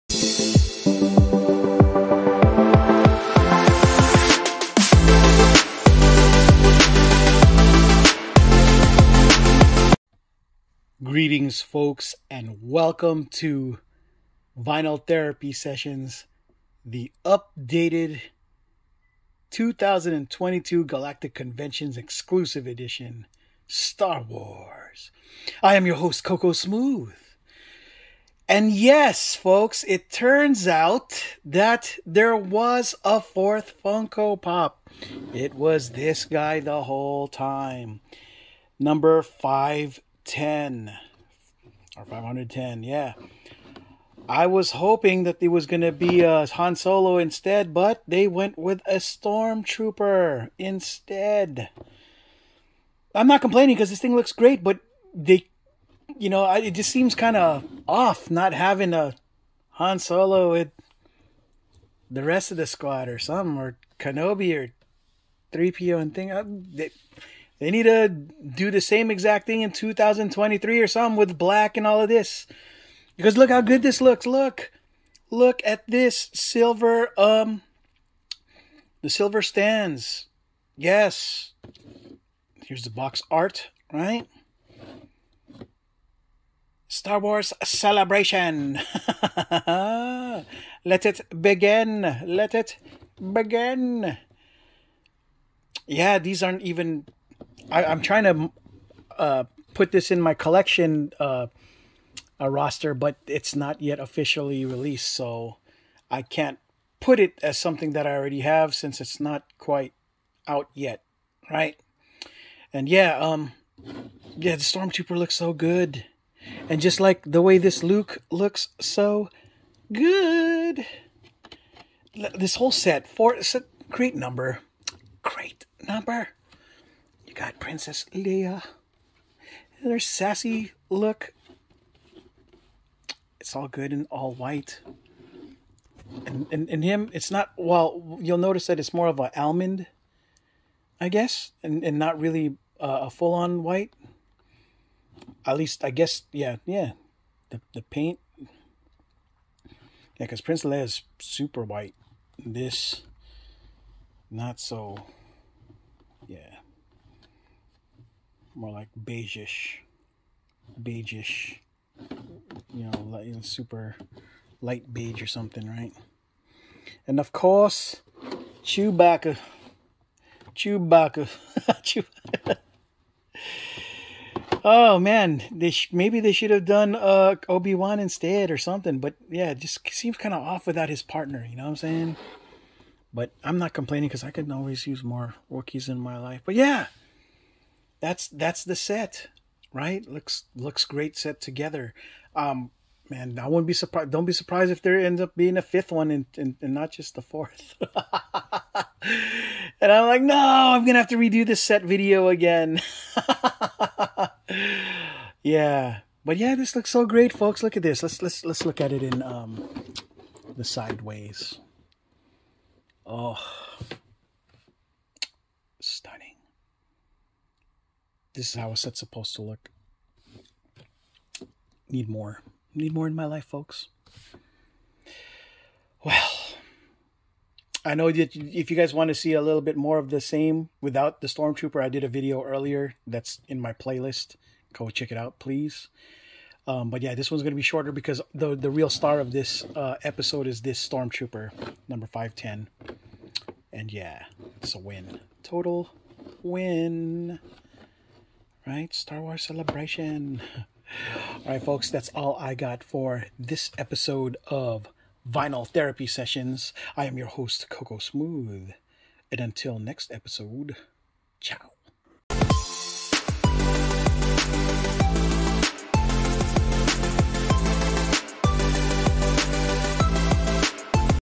Vinyl Therapy Sessions: Funko Pop! 2022 Star Wars Galactic Convention Exclusive (Complete Set)
Showcasing my Funko Pop! UPDATED 2022 Star Wars Galactic Convention Exclusive Set Star Wars Celebration Complete Set with Storm Trooper Luke Skywalker, Princess Leia, and Chewbacca